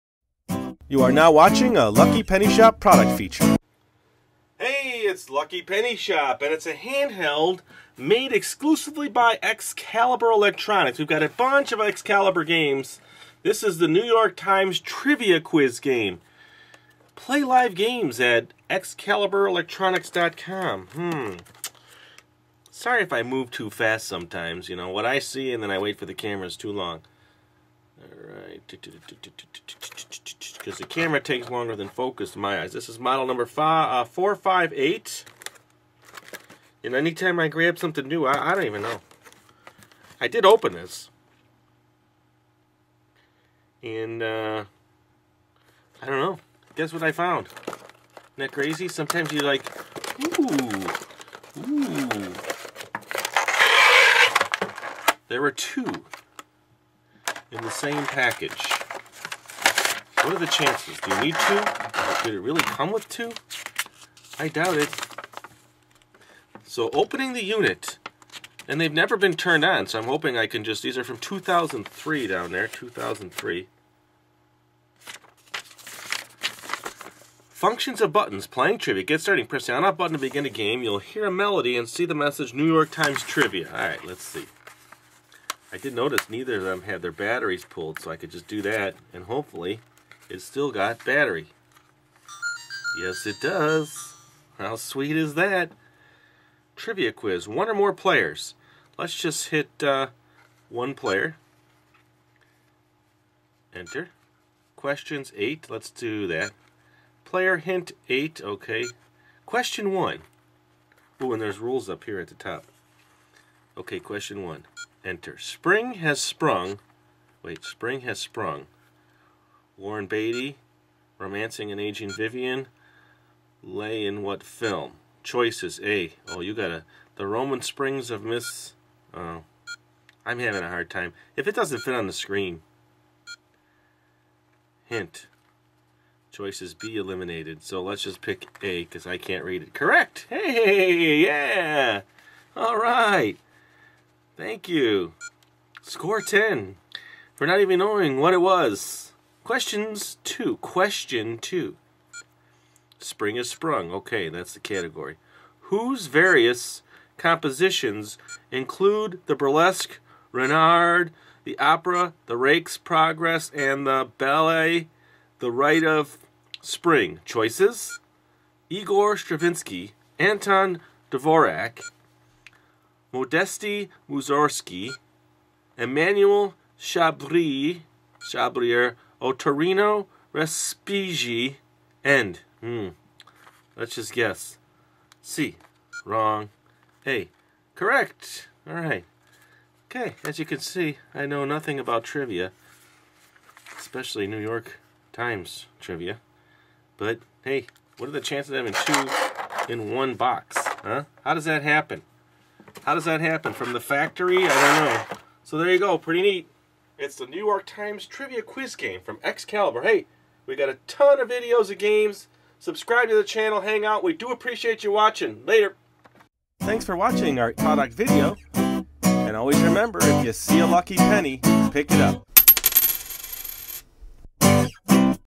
The New York Times Trivia Electronic Handheld Game, 2003 Excalibur Electronics
​​​Watch our product feature video for a The New York Times Trivia Electronic Handheld Game, 2003 Excalibur Electronics.